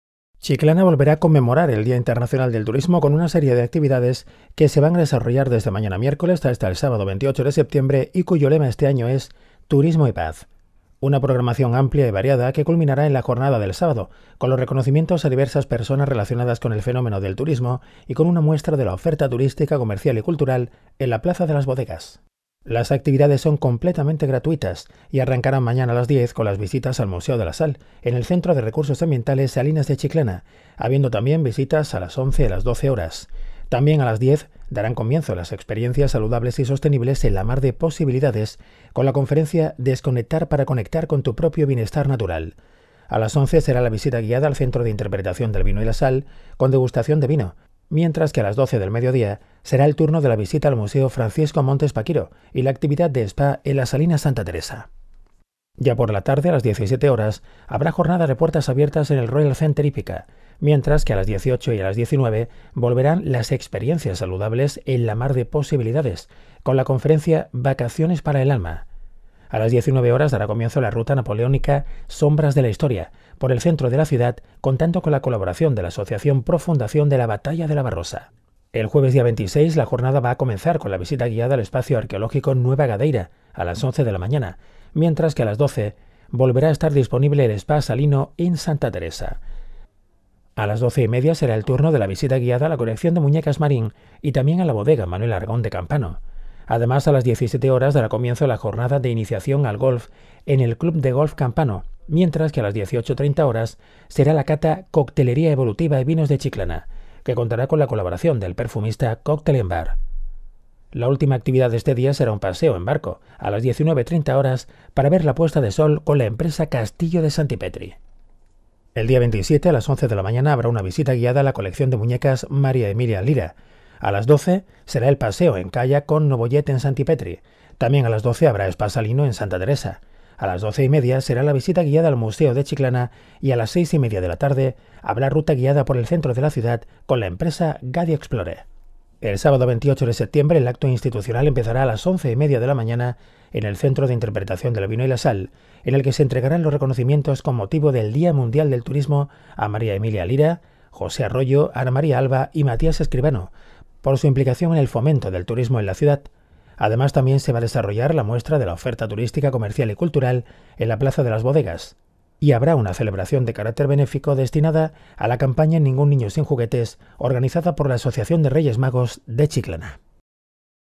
LAS ACTIVIDADES PARA CONMEMORAR EL DÍA MUNDIAL DEL TURISMO DARÁN COMIENZO MAÑANA EN CHICLANA
Las actividades para conmemorar el Día Mundial del Turismo darán comienzo mañana. En la jornada del sábado se celebrará el acto institucional en el que se reconocerá la labor de diversas personas por el fomento del turismo en la ciudad, a las 11.30 horas en el Centro de Interpretación del Vino y la Sal.